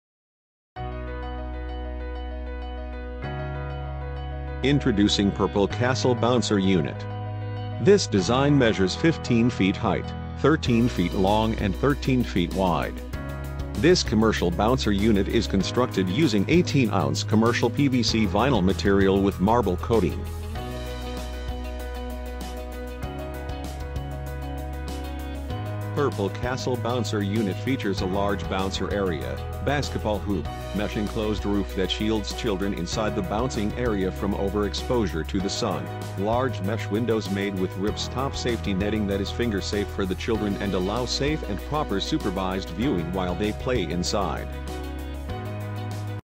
Purple Castle
Introducing Purple Castle Bouncer Unit. This design measures 15FT Height, 13 FT Long and 13 FT wide. This commercial bouncer unit is constructed using 18 -ounce commercial PVC vinyl material with marble coating. Purple Castle Bouncer unit features a large bouncer area, basketball hoop, mesh enclosed roof that shields children inside the bouncing area from overexposure to the sun, large mesh windows made with rips top safety netting that is finger-safe for the children and allow safe and proper supervised viewing while they play inside.